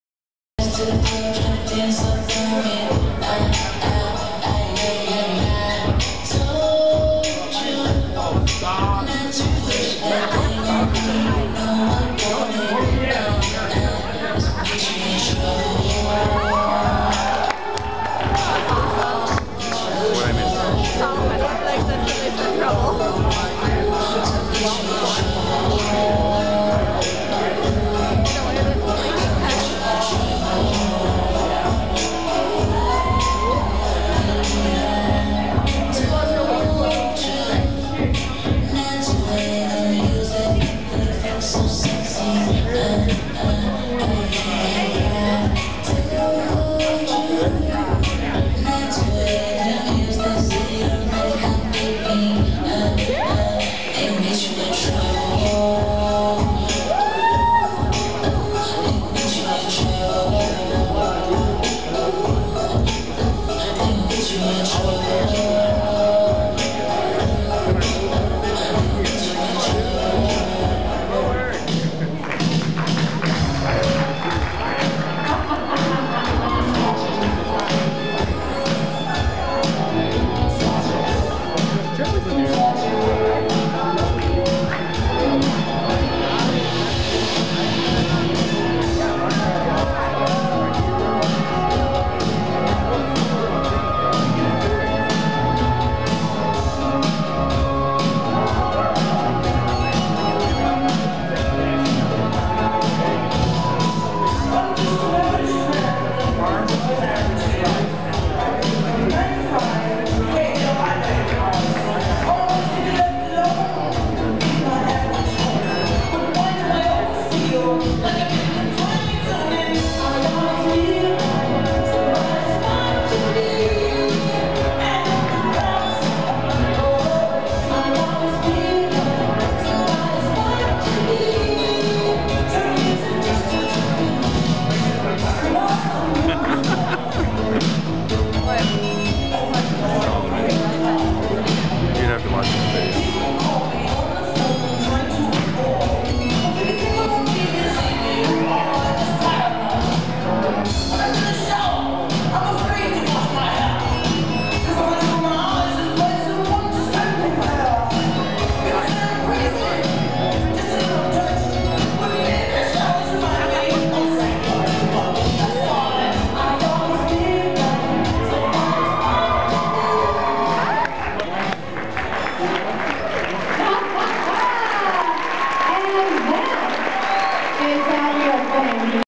Swing City Chicago 2012 All-star Jack and Jill Finals 1
Kris Swearingen, Melissa Moy, Soleman Krebs, Chelsea Spika